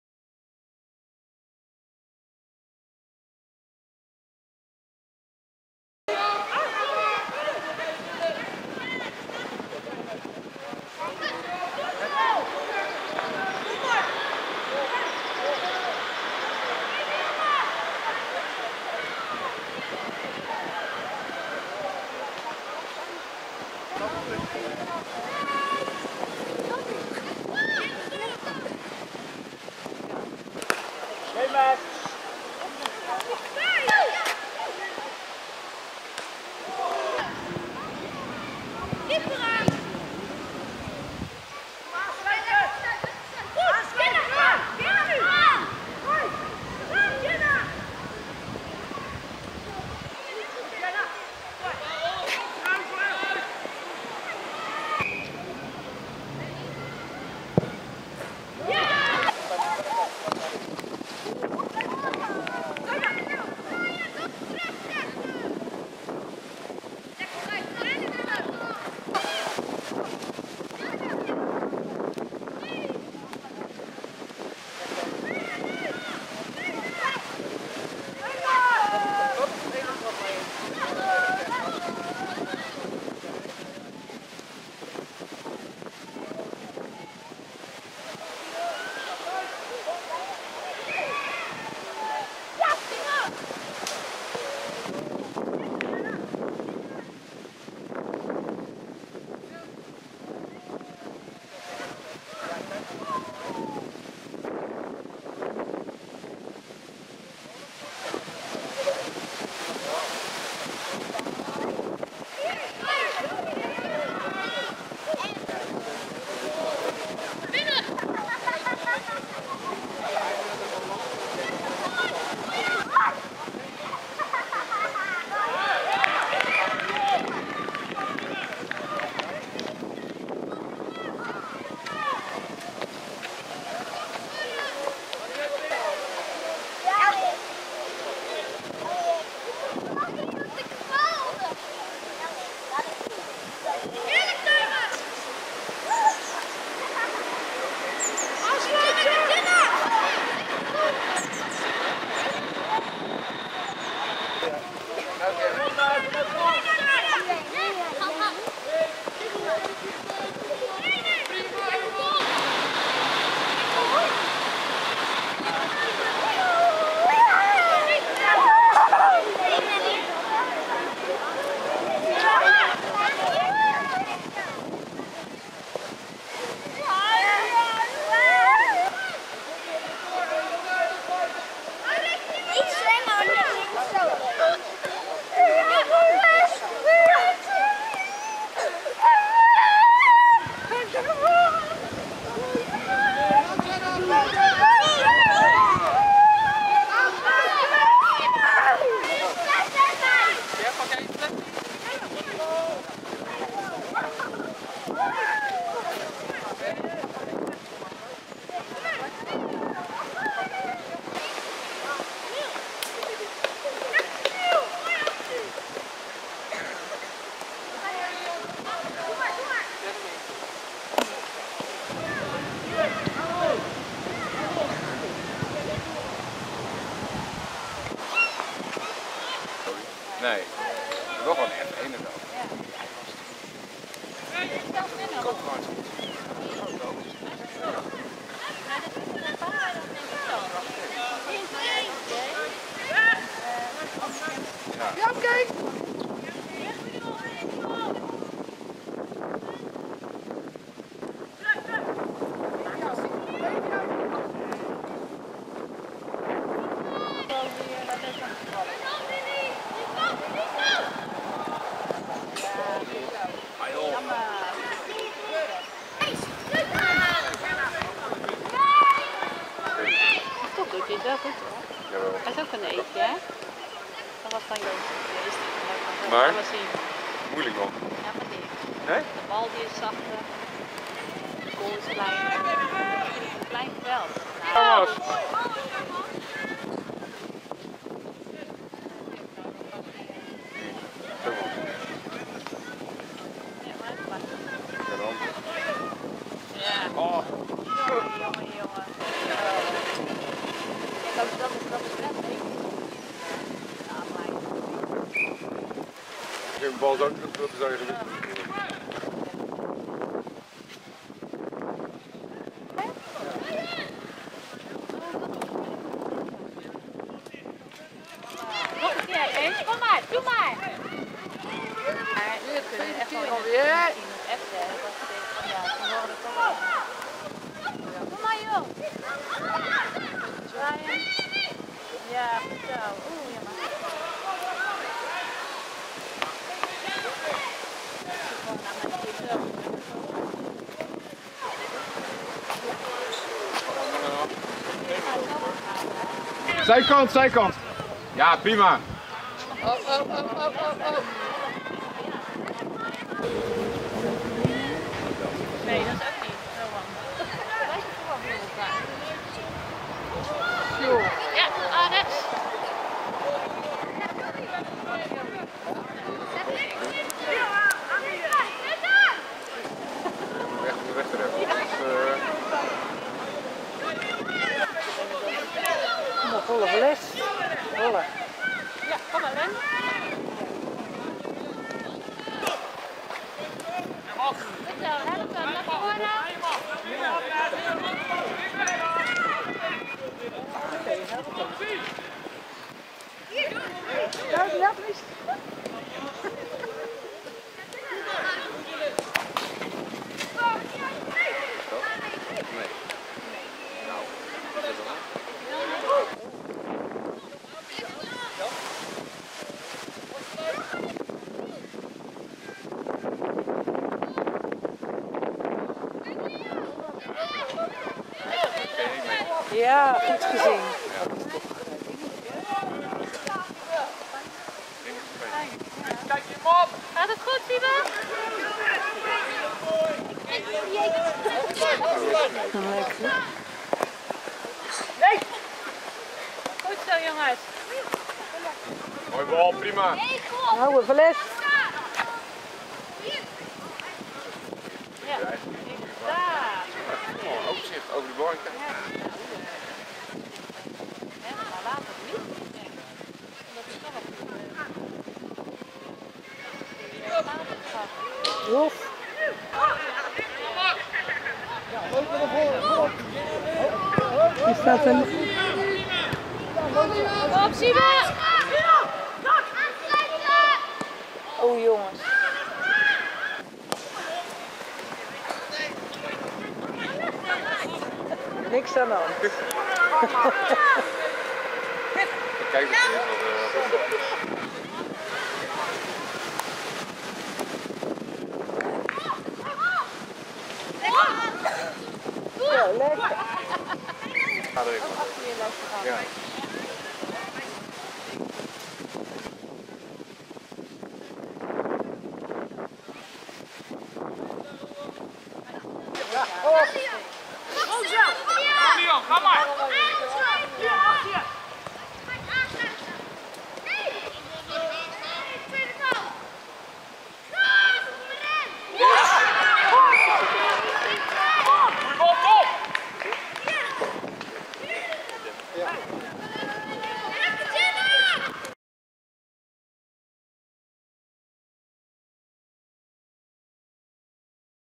Samenvatting PPSC D1 - Hermes DVS D nieuw
Oefenwedstrijd waarin de keurmeesters de spelers en speelsters kunnen bekijken, die komend seizoen in de D spelen van PPSC en Hermes DVS.